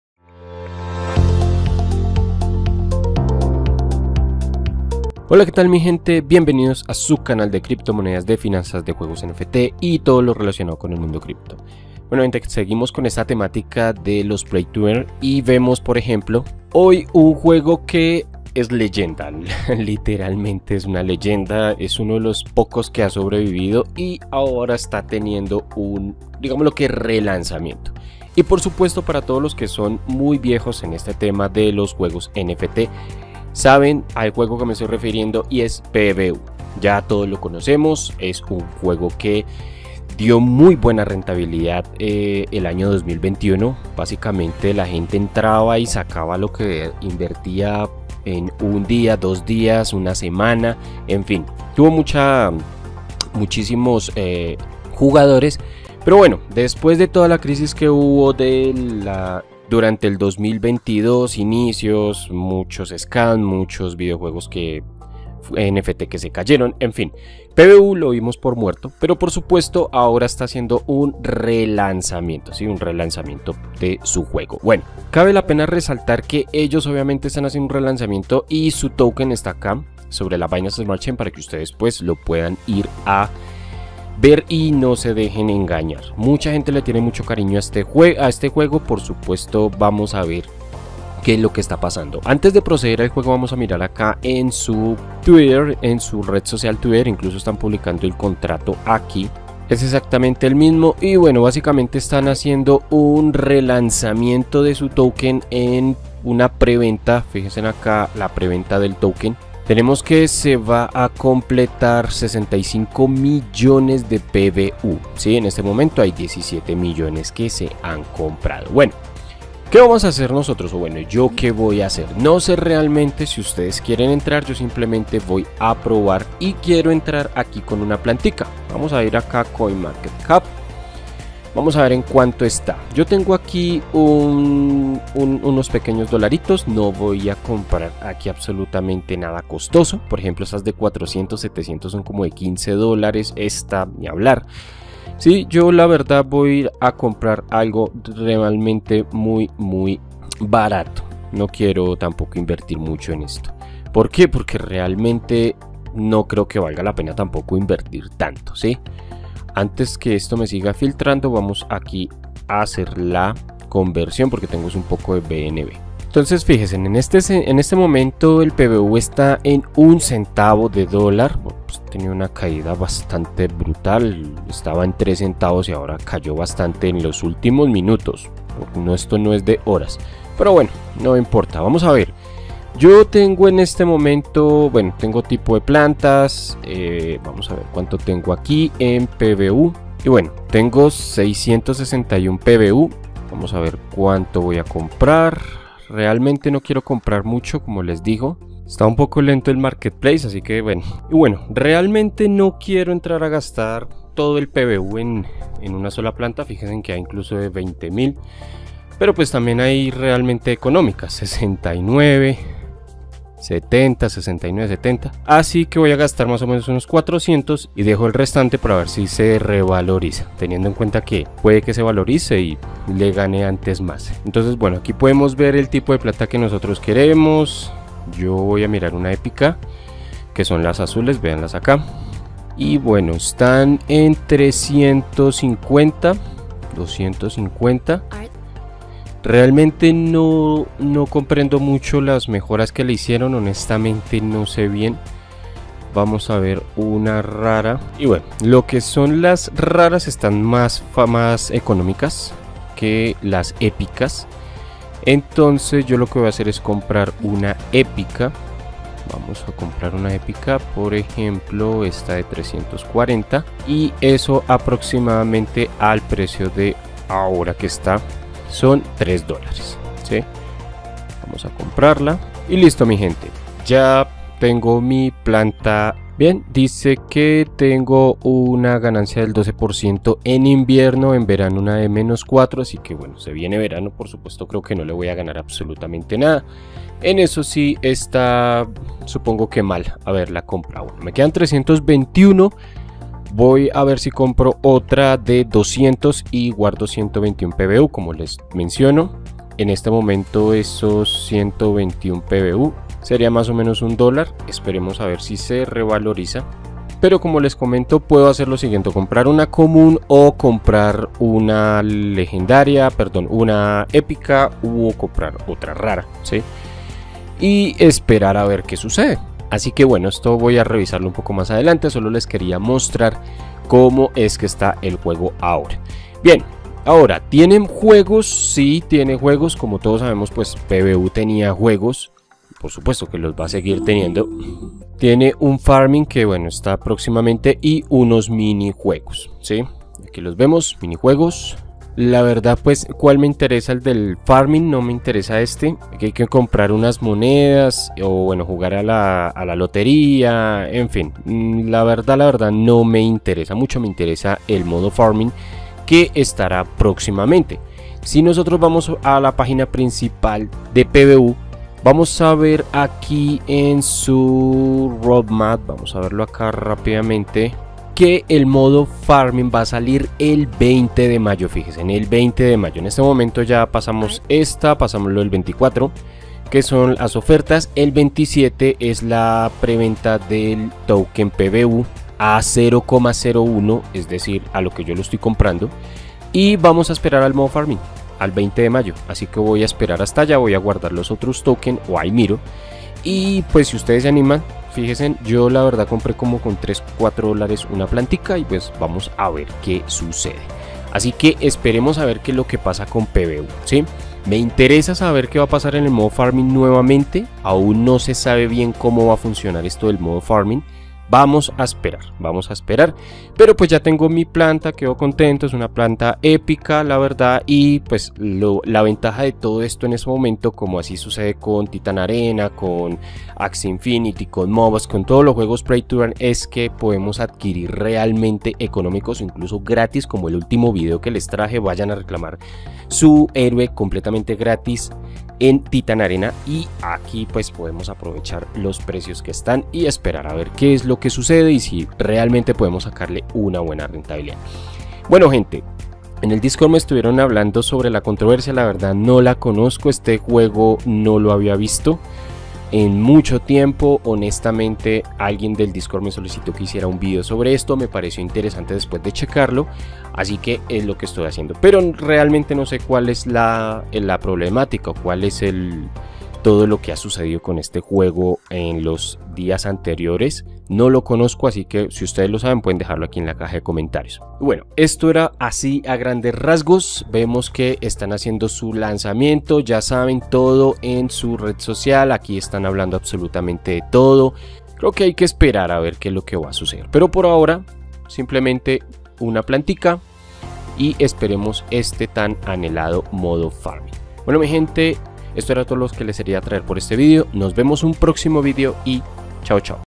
¿El renacer de un juego NFT leyenda?
Uno de los juegos NFT más queridos por los aficionados a los Play To Earn renace con su token a $0.01 USD y un mejor juego PVU